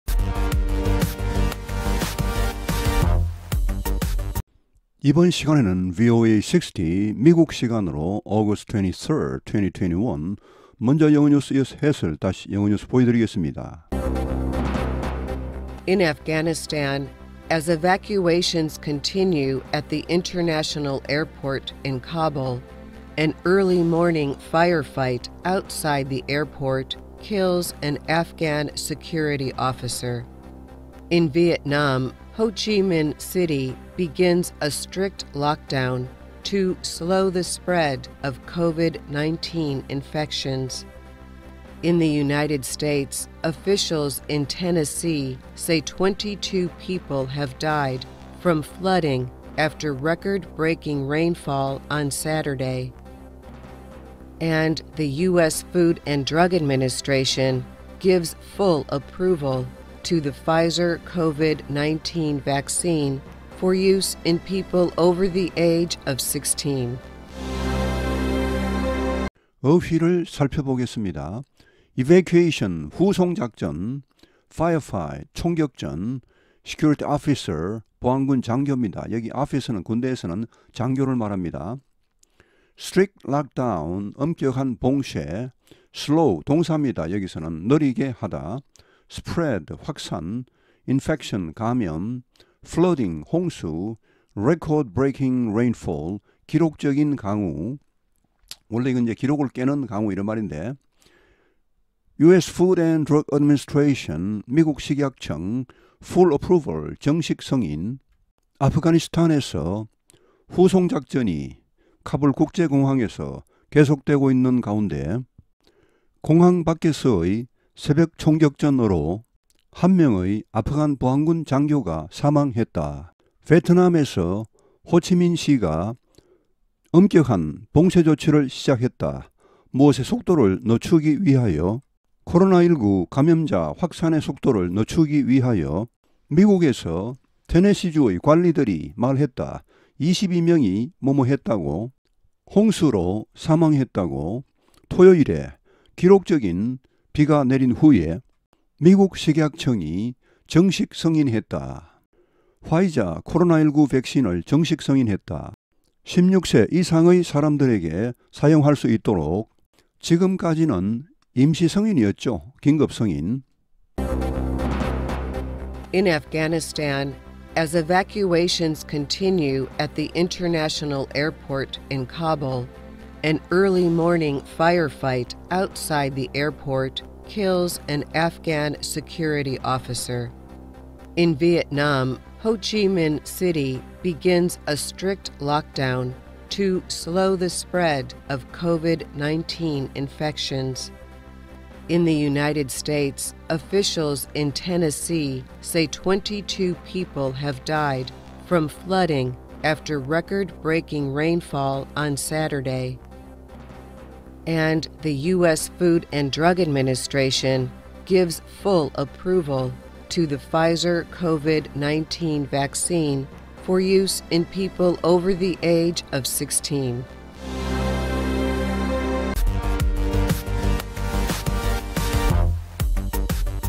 [VOA 영어뉴스 공부] 오늘자 톱뉴스 4개 (2021.8.24.화요일)
오늘의 세계 정세를 한 눈에 볼 수 있는 초소형 영어뉴스광장 및 우리말 해설입니다. 영어공부의 기초체력 및 영어내공 향상에도 좋습니다. 앞으로 좋은 일이 많이 있을 것입니다.

1. 경력

2. 저서 (책표지에 지은이로 실림)